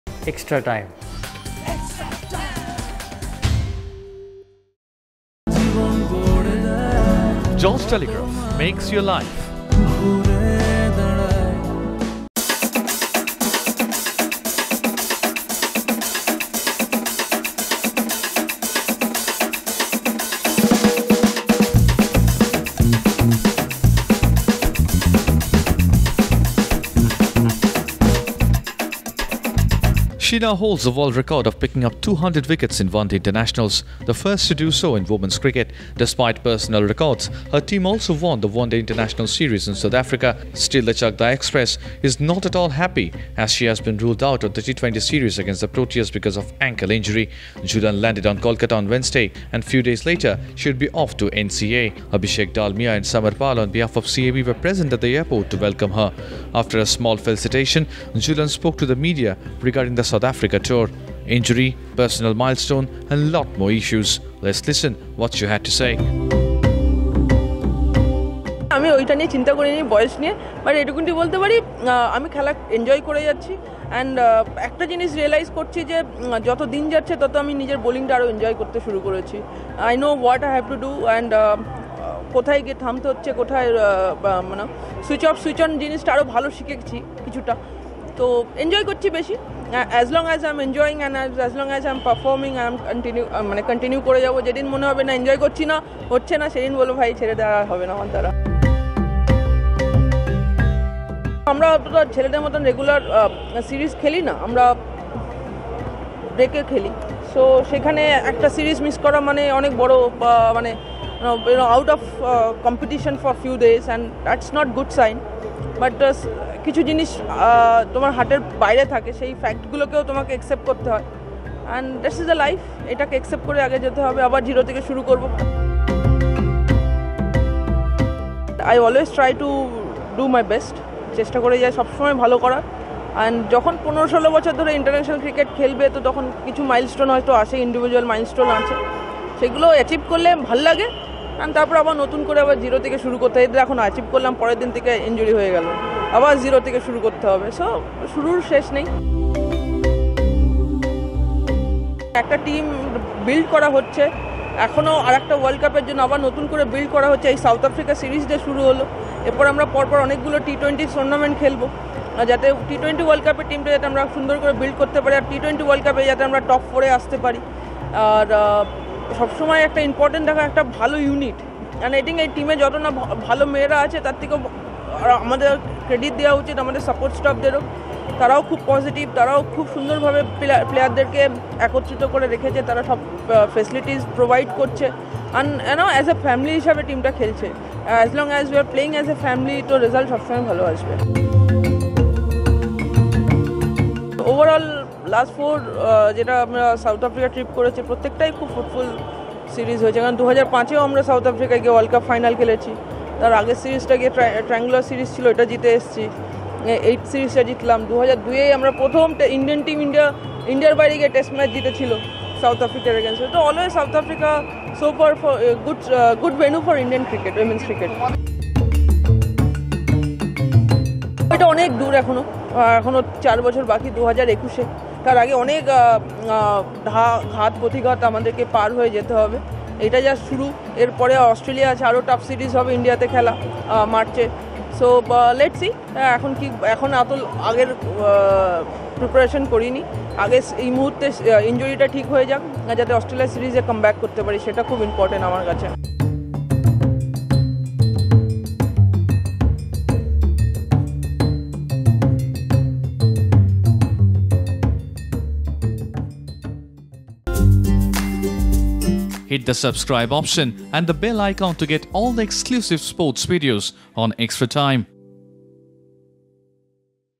Watch: Jhulan Goswami opens up about her historic 200th wicket in ODI cricket
She now holds the world record of picking up 20p wickets in One Day Internationals. The first to do so in Women's cricket. Despite personal records, her team also won the ODI series in South Africa as well. Still the Chakdah Express is not happy as she has been ruled out of the T20 series against the Proteas because of ankle injury. Jhulan landed on Kolkata on Wednesday. Few days later, she would be off to the NCA